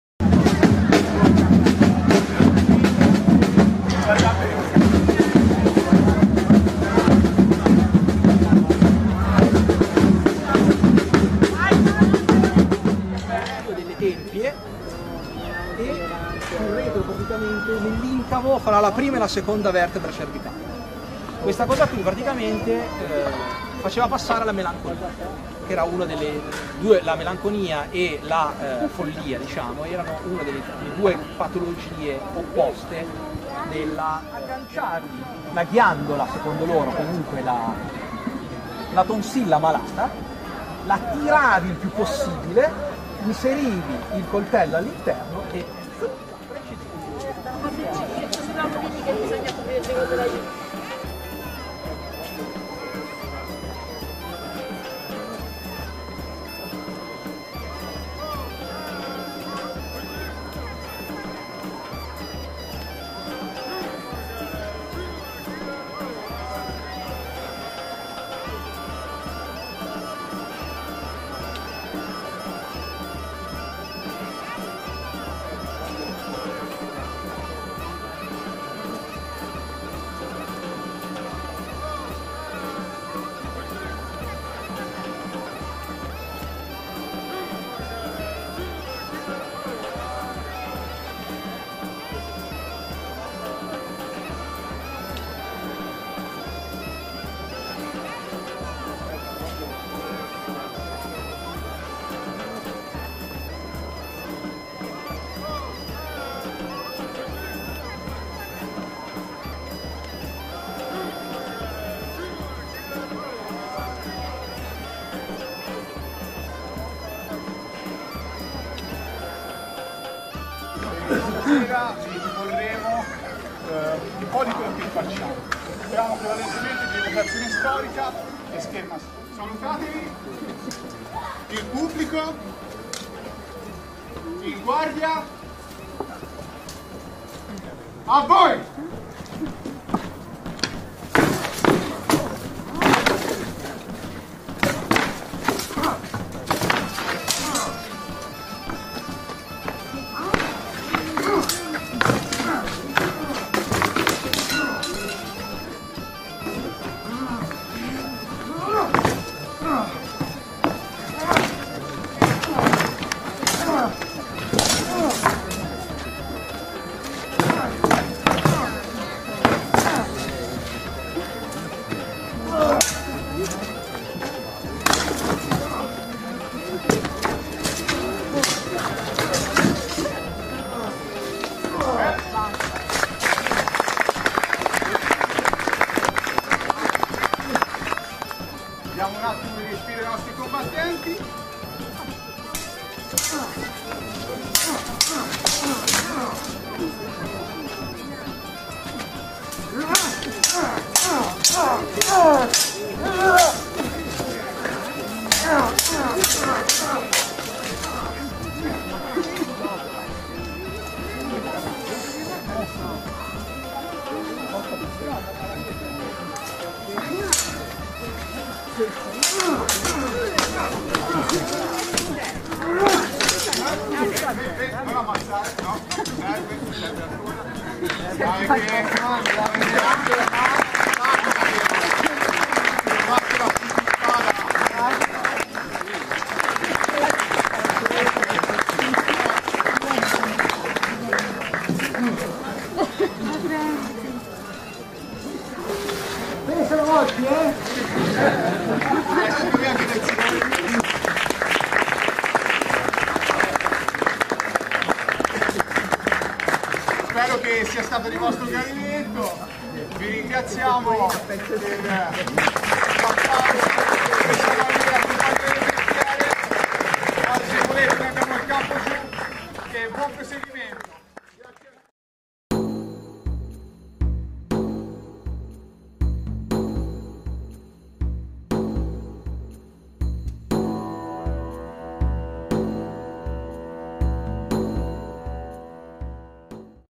#2 giorno - VICOPISANO MEDIEVALE - ed 2018
Un estratto del secondo giorno alla festa medievale di vicopisano.